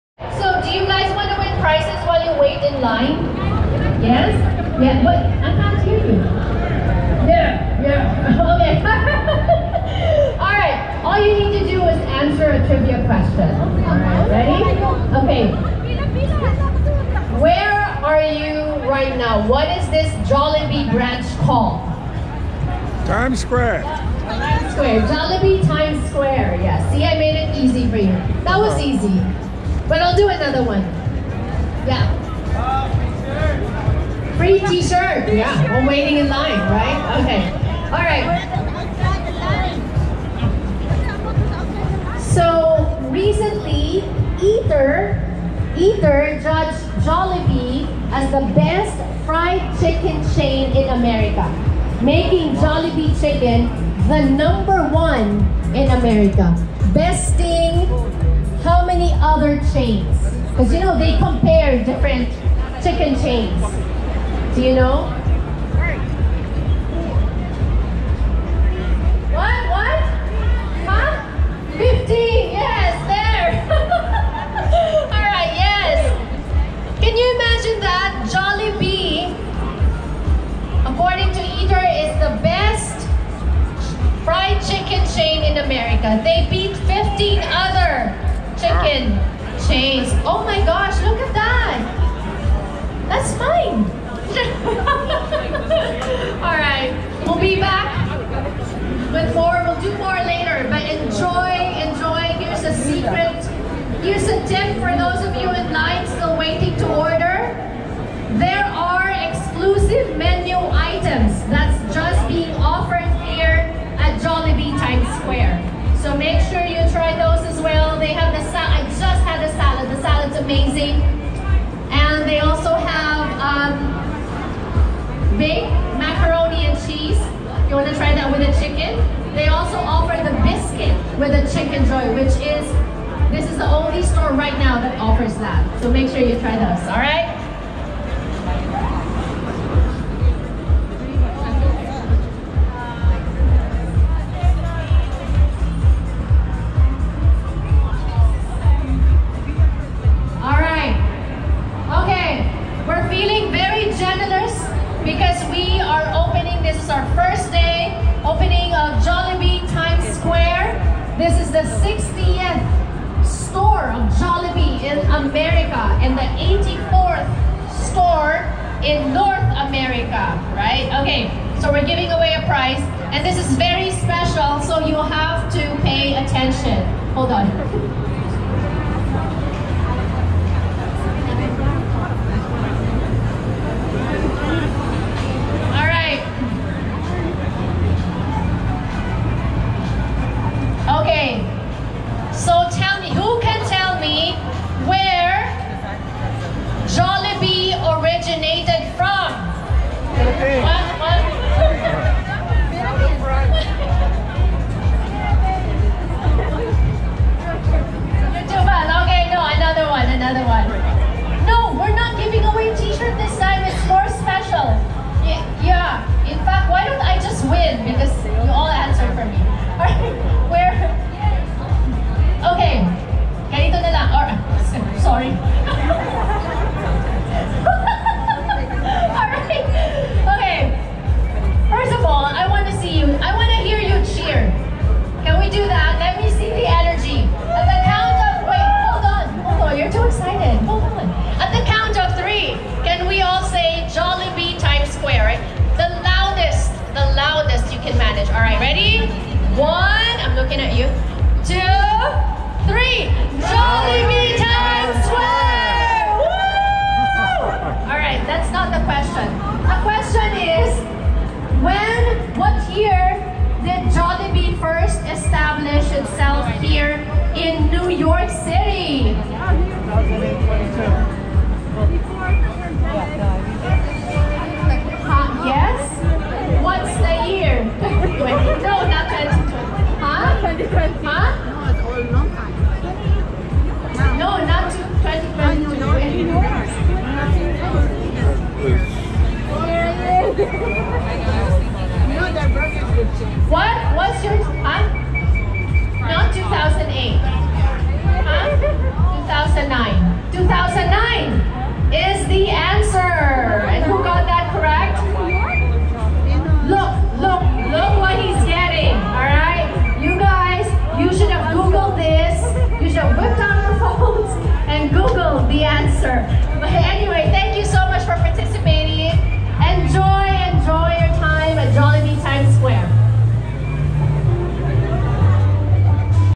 Jollibee Times Square Grand Opening Part 1 of 7
Jollibee had a grand opening in Times Square today. This is significant. This area gets lots of foot traffic. It's right across from the NYPD building. They sure will make a lot of money and are in the right place to be safe.

In this video we get a bit of history regarding when Jollibee first opened up in the US. It's also noted that Jollibee is #1 for chicken taste out of 15 chicken establishments. So that is something to brag about.